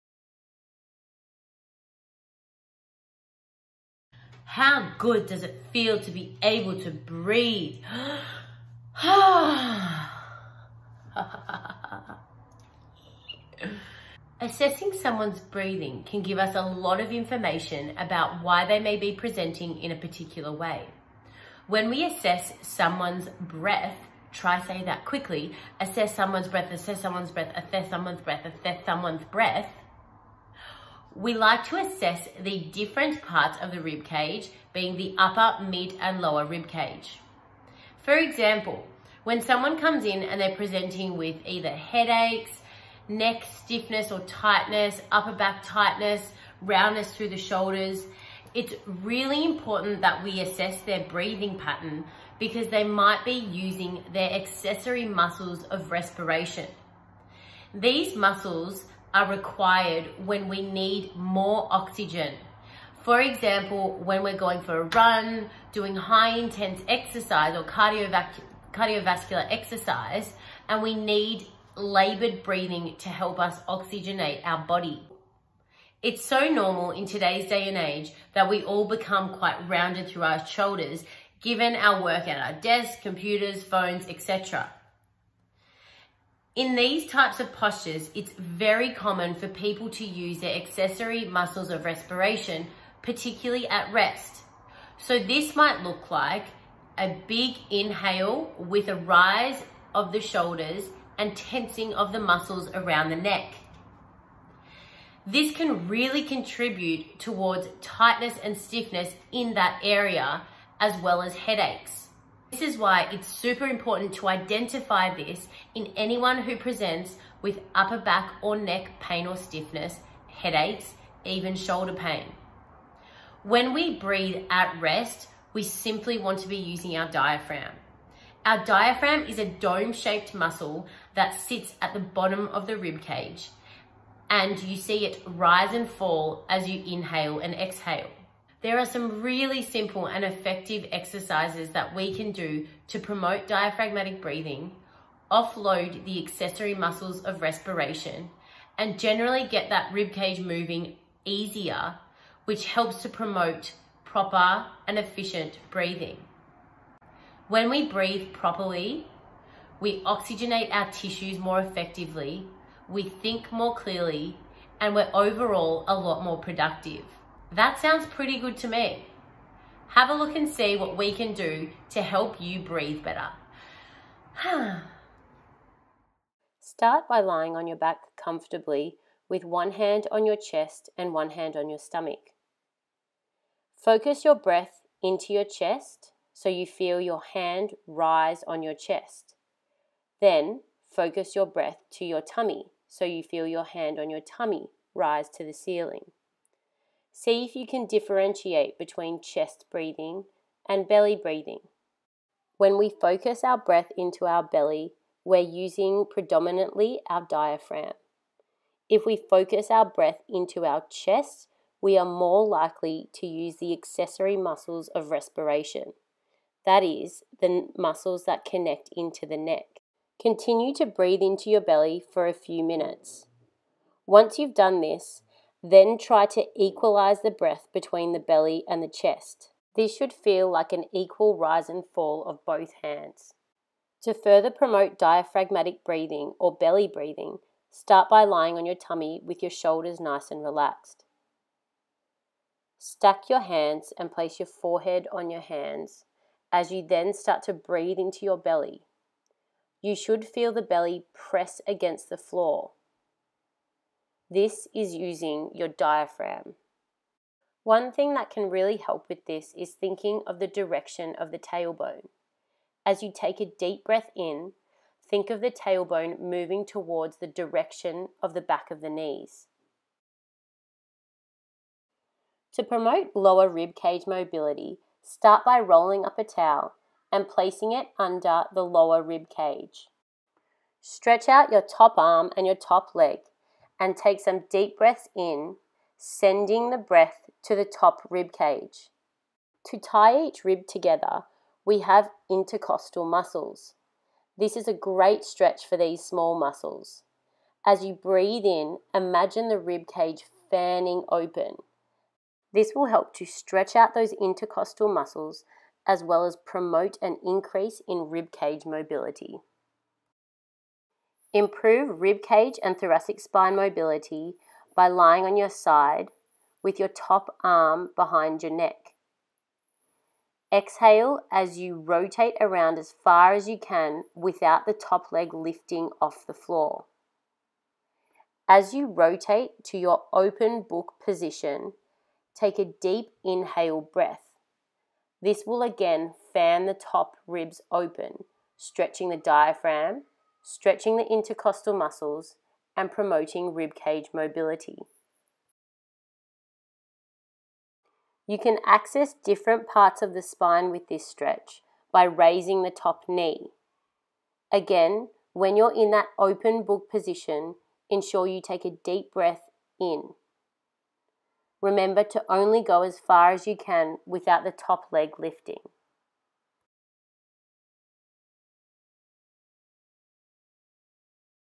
HOW TO: Breathe
It's something we all do. It's something we all need. It's something we can all be better at. Although it is autonomous, breathing is something we can always have control over - it just takes practice!

The way someone breathes can also give us a lot of information as to why they may be presenting a particular way in their musculo-skeletal system.

In this video we discuss the importance of breath, in addition to some really simple exercises you can do to help you breathe better and feel better!

For more great content, head to the blog section on our